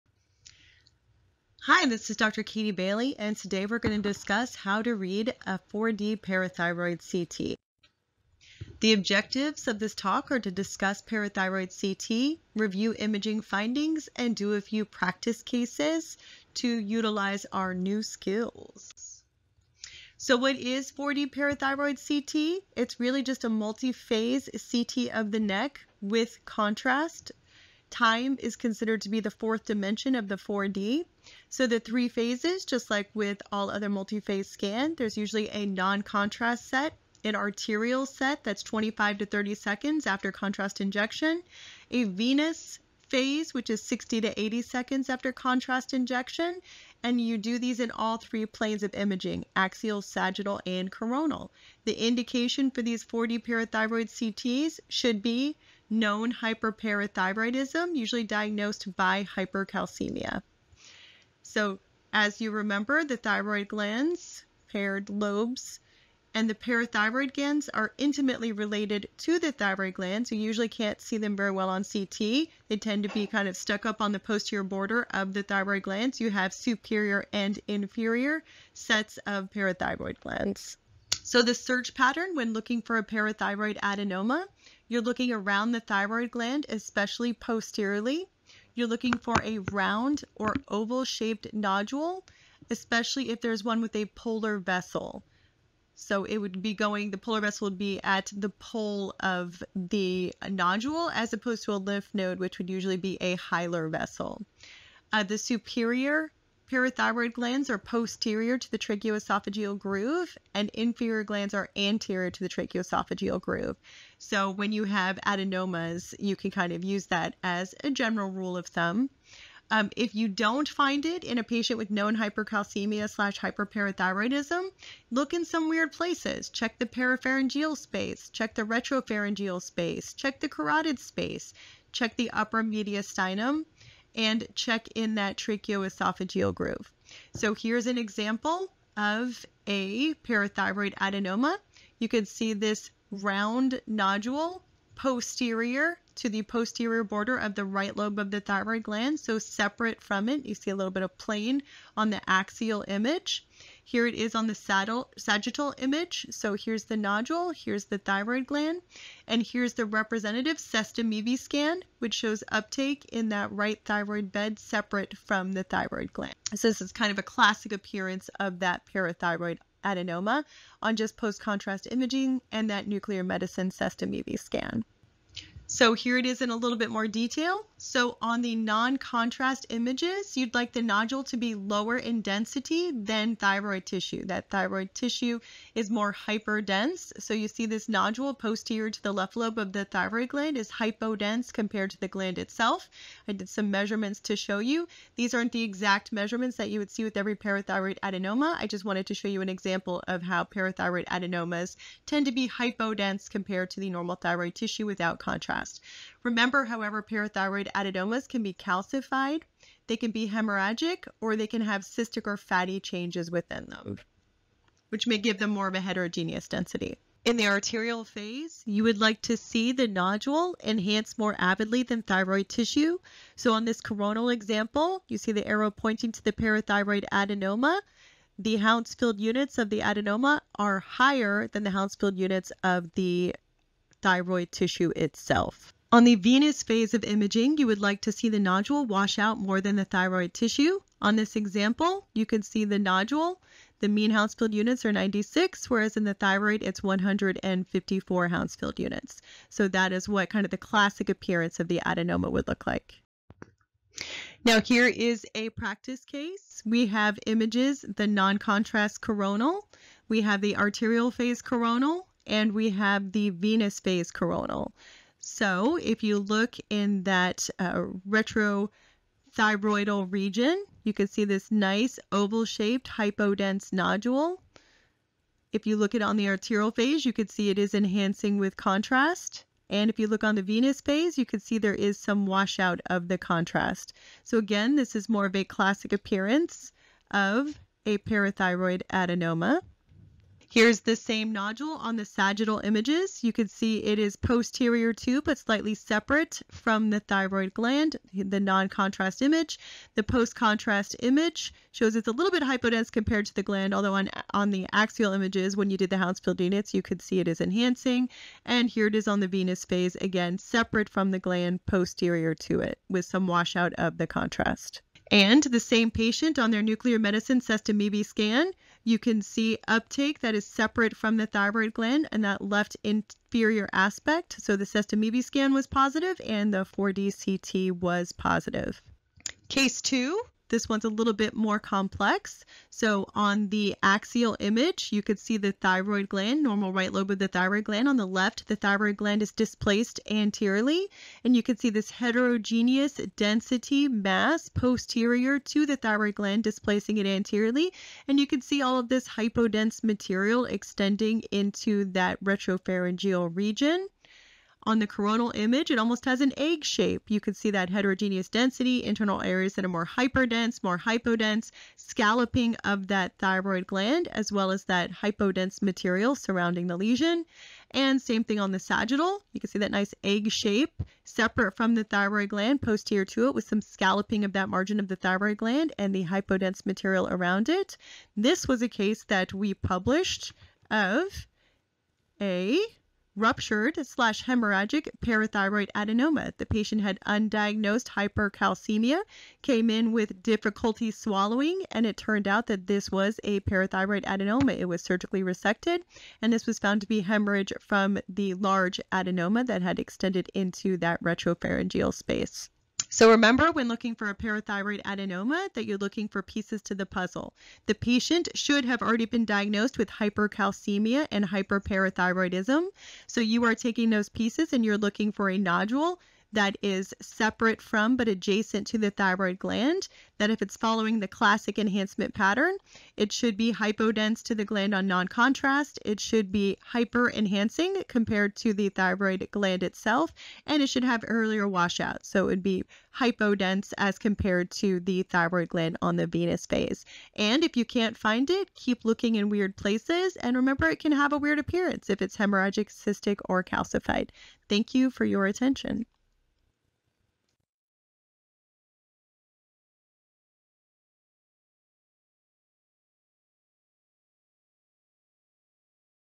How to read a 4D parathyroid CT
In this video, Dr. Katie Bailey describes how to review a 4D parathyroid CT for the presence of a parathyroid adenoma. We cover the features of a multiphase, or 4D, CT of the neck and the imaging characteristics which are typical of parathyroid adenomas.

0:00 Introduction

0:19 What is 4D parathyroid CT?
It is a multiphase contrast CT, so you have three dimensions in space, and the 4th dimension is time. It is often done to evaluate for the possible presence of abnormal parathyroid glands or a parathyroid adenoma. There are usually 4 parathyroid glands along the posterior margin of the gland.

1:20 How to find a parathyroid adenoma
If looking for a parathyroid adenoma, you should be looking for a round or oval nodule along the posterior margins of the thyroid. A good way to differentiate between a lymph node and a parathyroid is that the vessel often goes to the pole of a parathyroid but to a hilum of a lymph node. If you don’t find one in the expected location, you can check more distant places in the deep neck.

2:53 Appearance of parathyroid adenoma
On a noncontrast image, the nodule should be lower in density than the normal thyroid, which contains more iodine.

On arterial phase imaging, a parathyroid adenoma enhances more than the adjacent thyroid.

On venous phase imaging, the adenoma enhances less than the adjacent thyroid because the contrast washes out faster.

4:11 Practice case 1
In this practice case, you can see a nodule along the left inferior tracheoesophageal groove. It enhances avidly on the arterial image and washes out on the more venous phase. The sestamibi scan confirms the finding.

5:26 Practice case 2
This case shows a normal thyroid which is displaced anteriorly on the left. There is a heterogenous nodule along the posterior margin and extending along the esophagus. The greater heterogeneity of this lesion is caused by hemorrhage

6:39 Summary
4D CT is a focused tool to look for parathyroid adenomas in the setting of a clinically suspected adenoma, usually characterized by hypercalcemia. You can use it in conjunction with clinical features, nuclear medicine

Be sure to check back in for additional videos in the future or check out the website at